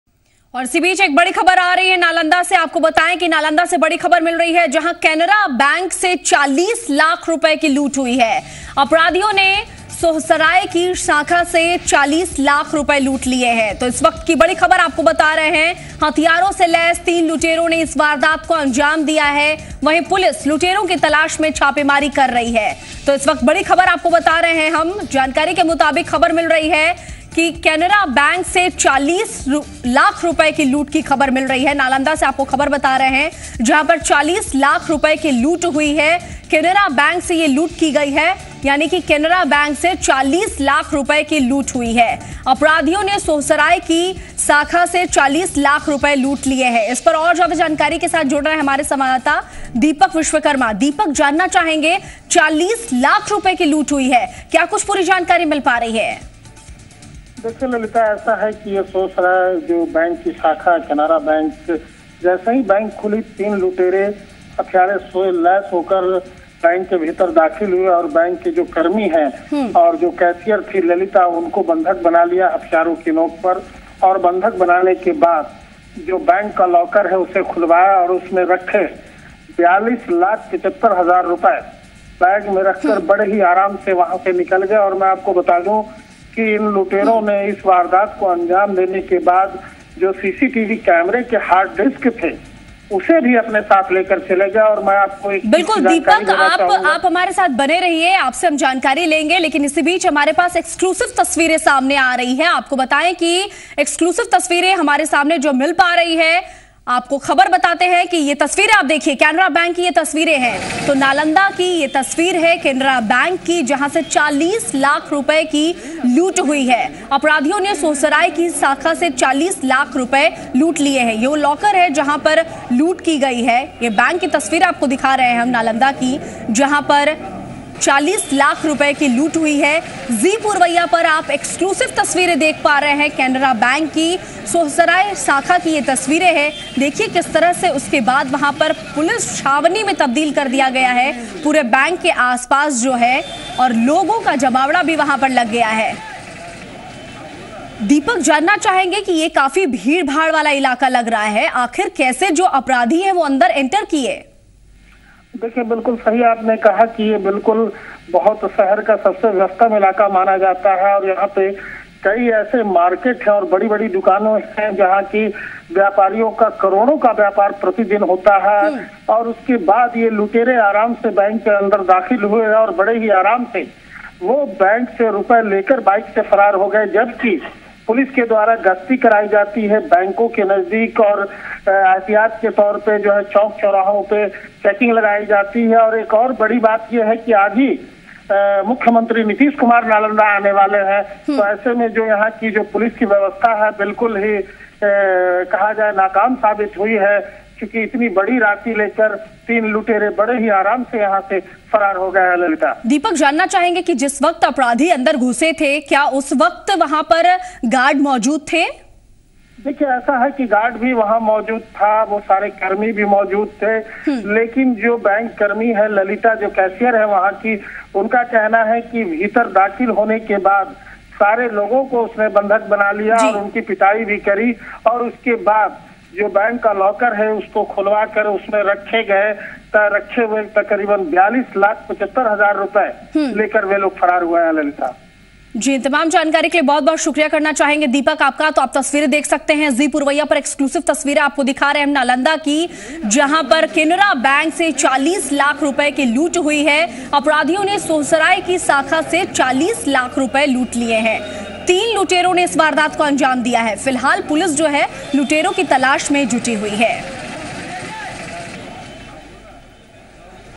Watch: Breaking News - Canara Bank Robbery In Nalanda
Canara bank in Nalanda has been robbed to a sum of Rs. 40 Lakhs.